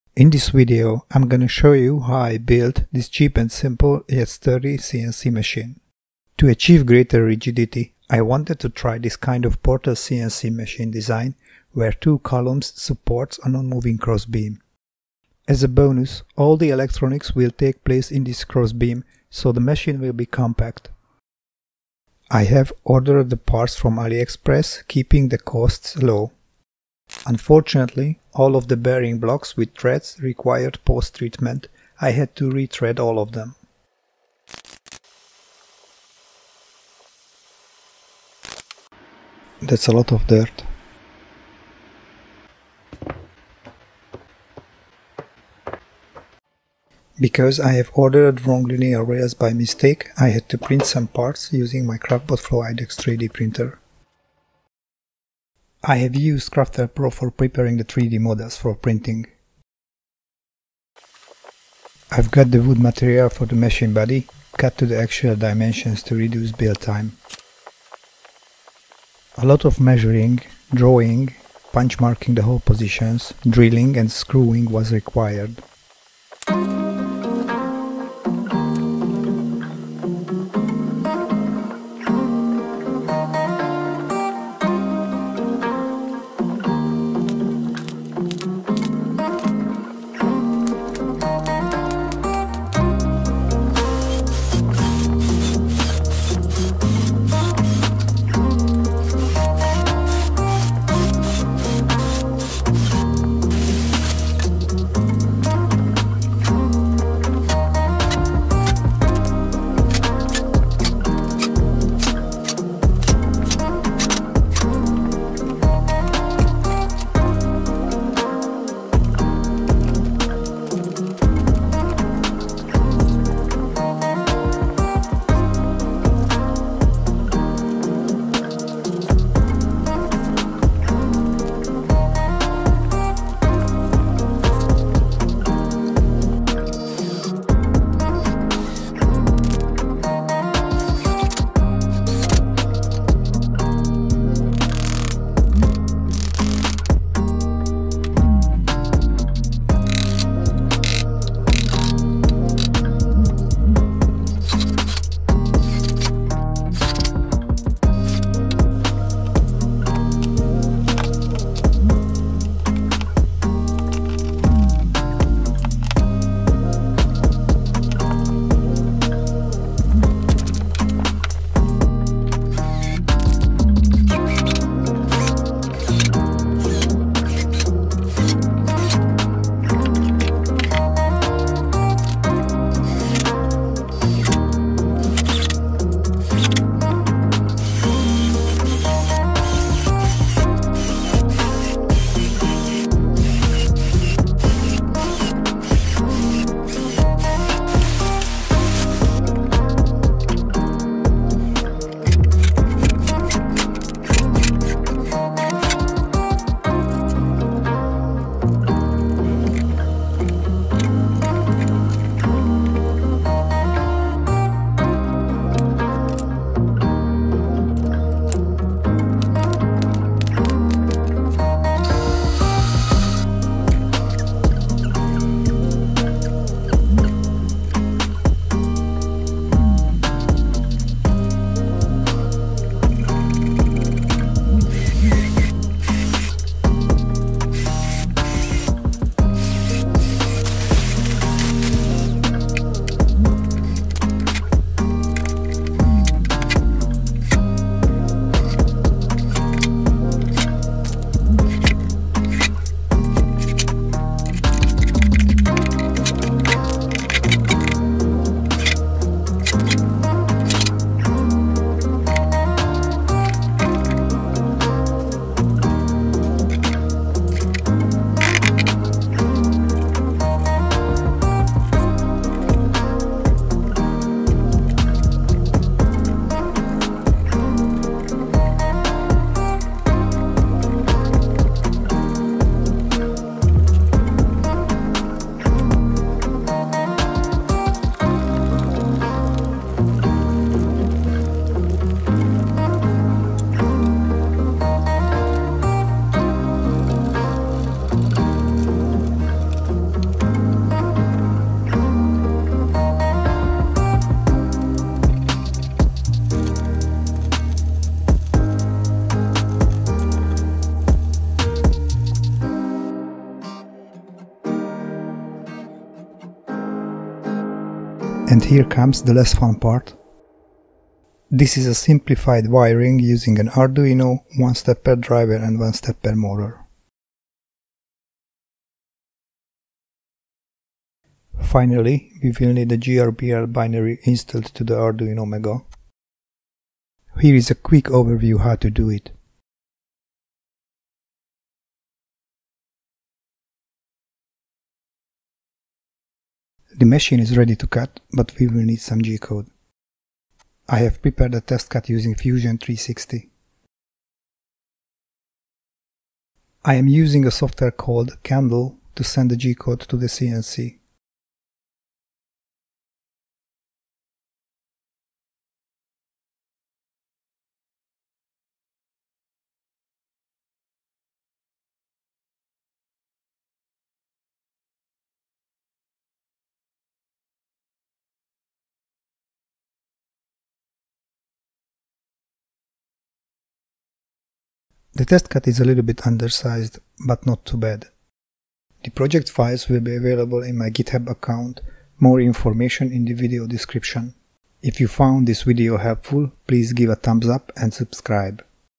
DIY CNC router build
Low cost wood CNC design. Complete build & guide using Arduino GRBL.